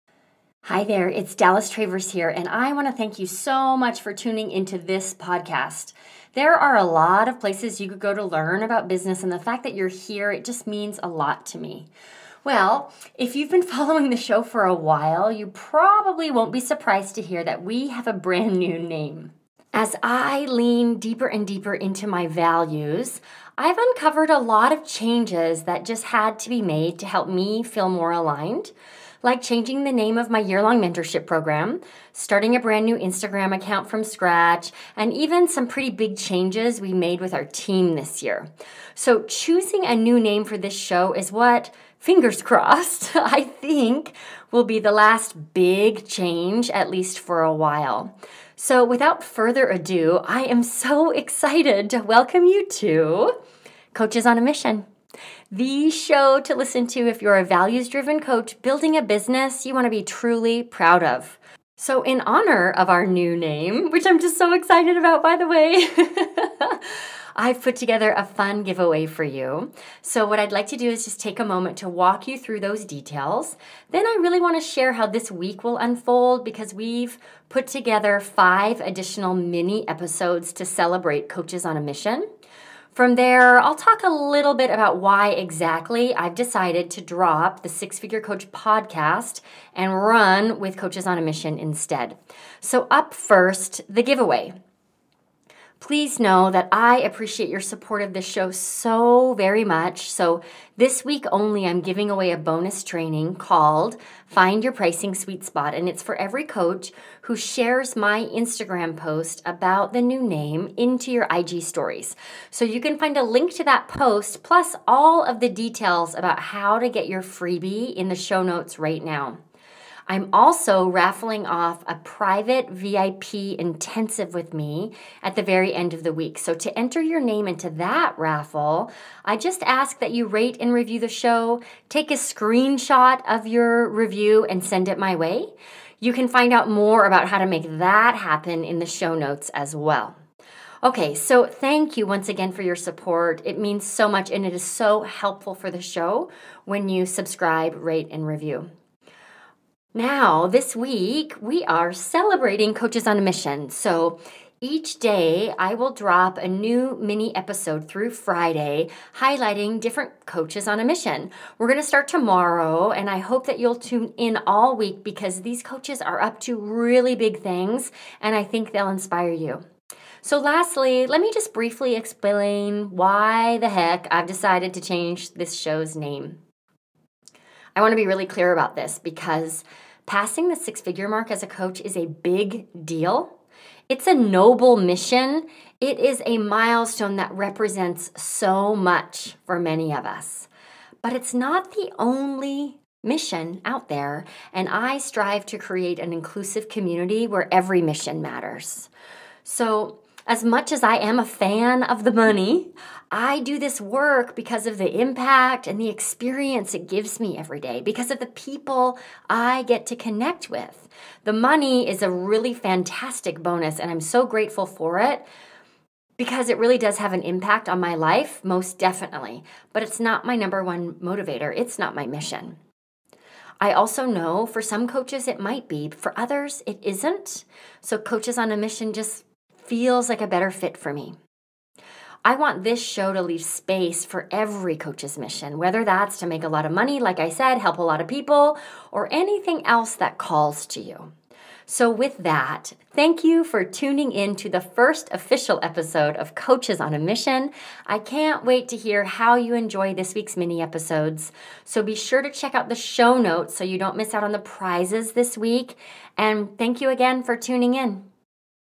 EP76 WELCOME TO COACHES ON A MISSION
If you’ve been following the show for a while, you probably won’t be surprised to hear that we have a brand new name.

As I lean deeper and deeper into my values, I’ve discovered a lot of changes to make to help me feel more aligned in my business and my life.

Like changing the name of my year long mentorship program, starting a brand new instagram account, and even some changes we’ve made with the team this year.

And, choosing a new name for this show is what, I think, will be the last big change… at least for a while.

So, without further ado, I am so excited to welcome you to Coaches on a Mission — THE show to listen to if you’re a values-driven coach looking to build a business you’re truly proud of.

In honor of our new name, I’ve put together a fun giveaway for you. And in this special episode, I will take a moment to walk you through those details, share a bit about how this week will unfold, and of course, spill the tea on why, exactly, I’ve decided to drop The Six Figure Coach Podcast and run with Coaches on a Mission instead.